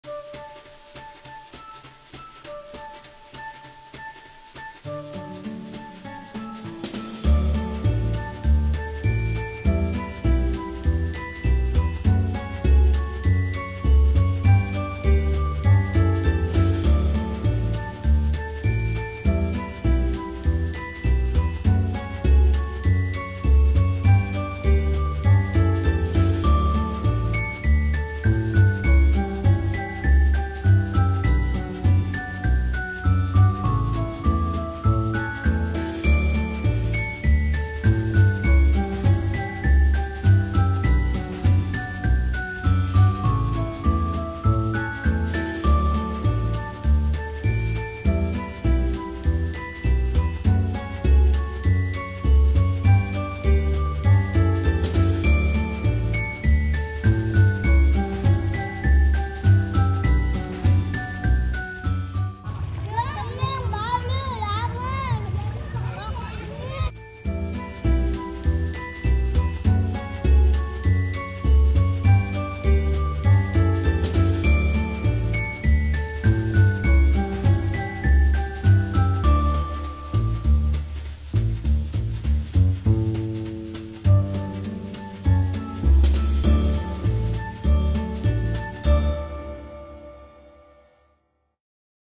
印尼龍目島Lombok, Indonesia
龍目島是峇里東邊的一個小島！相對較峇里遲開發，景點和環境與峇里有點像，但遊人較少！